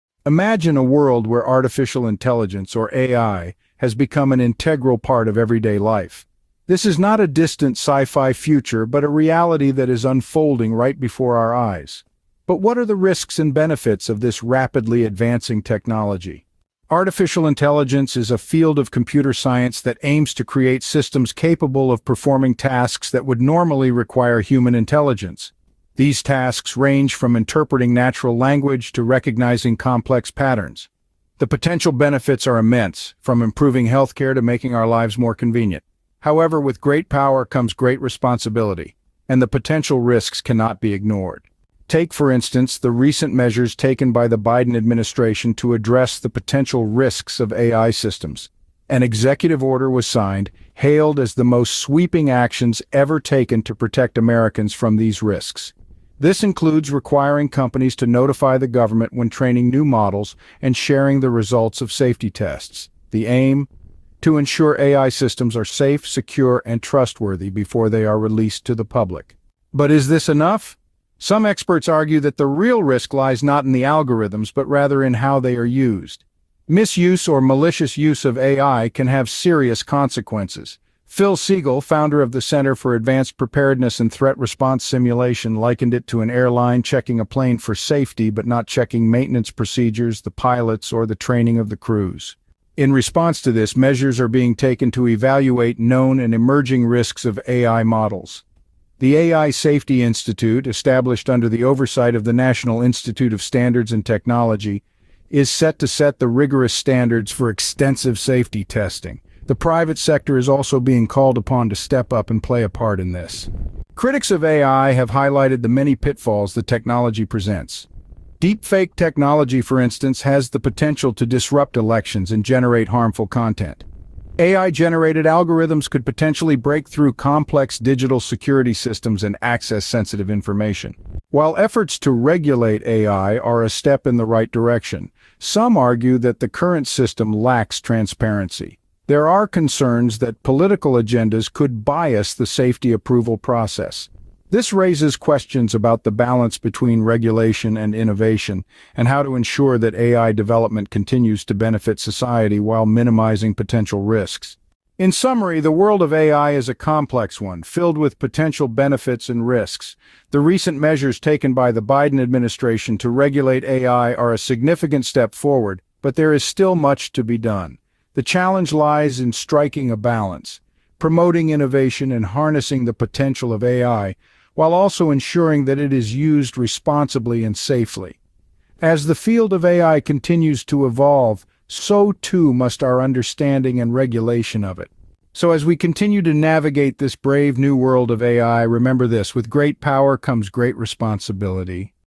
AI Safety Commission: A Necessary Step Or Not Enough?
Experts say the Biden administration’s plan to establish an artificial intelligence (AI) safety commission may prove "necessary" but not "sufficient" to address potential risks for the burgeoning technology.
The administration also announced the establishment of the AI Safety Institute – under the oversight of the National Institute of Standards and Technology – which will "set the rigorous standards for extensive red-team testing to ensure safety before public release."
"The odds are the algorithm is not where the majority of the risk lies," said Phil Siegel, founder of the Center for Advanced Preparedness and Threat Response Simulation (CAPTRS). "It is more likely the risk lies in the users either using it for bad or just plain misusing it."

00:00:00
Introduction to AI

00:00:16
Benefits of AI

00:00:41
Addressing the Risks

00:01:09
The Real Risk

00:01:32
Evaluating Risks

00:01:53
Pitfalls of AI

00:02:11
Transparency and Regulation

00:02:34
Conclusion

00:03:05
Final Thoughts